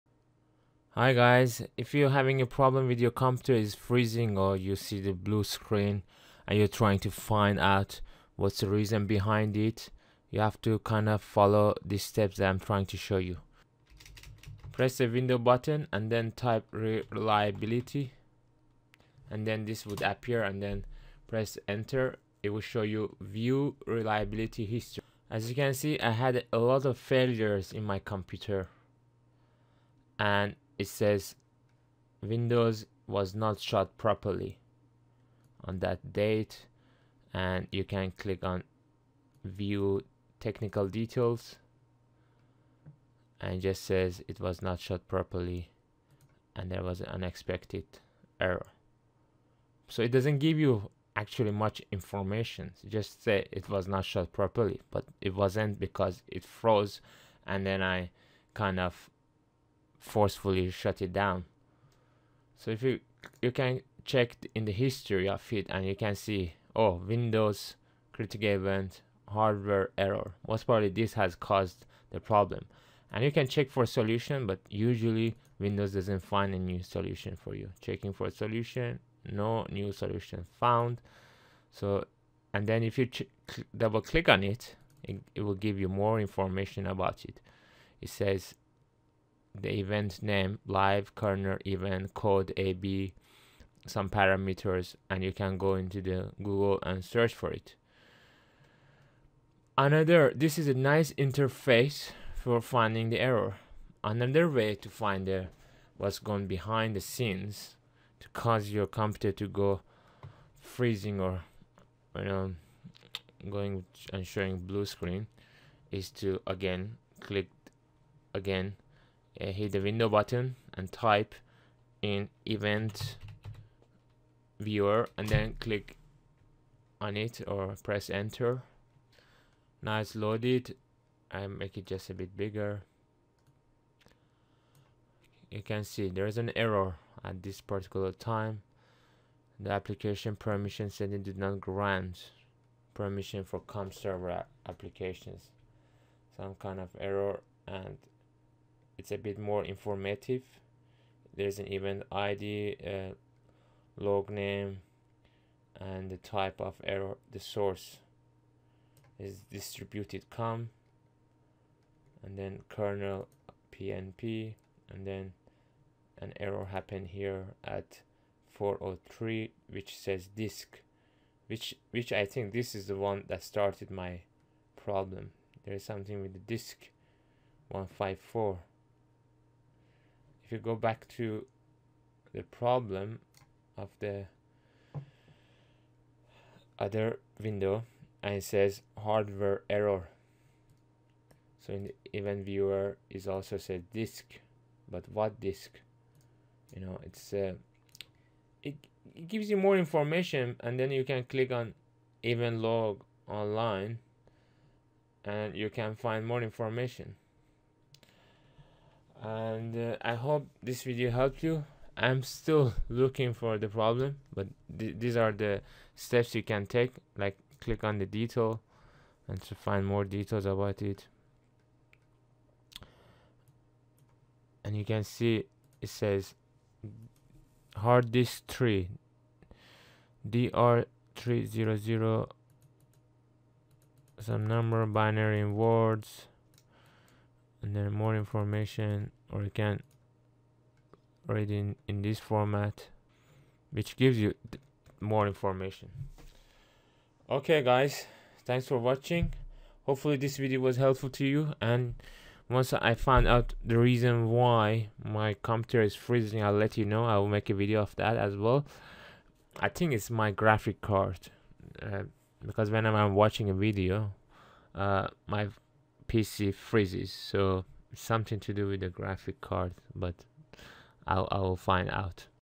find out why your windows froze or crashed
In this video, i'll show you some useful tools to find what happened behind the scences that led to windows to crash, freeze etc. These very useful tools that you'll need them at some point in the future so please bookmark this video for your future references.